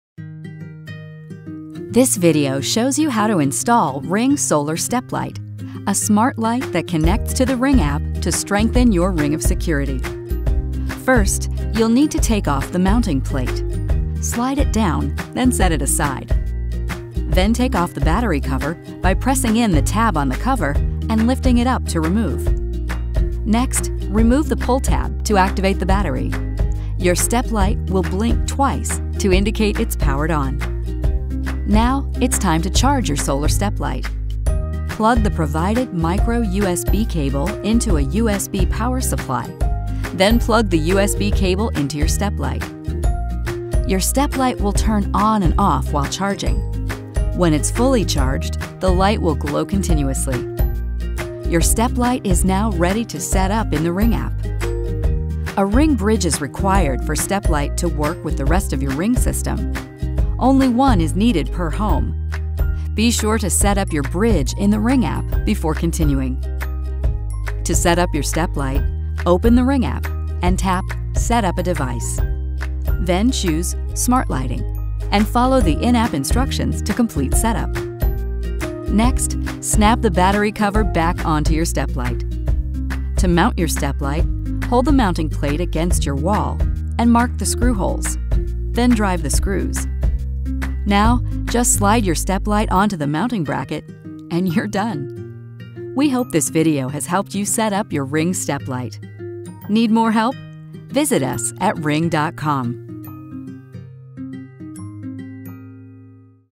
How to Install Ring Steplight Solar
Learn how to install Ring Steplight Solar, a smart, solar-powered outdoor light that illuminates your property and monitors for motion.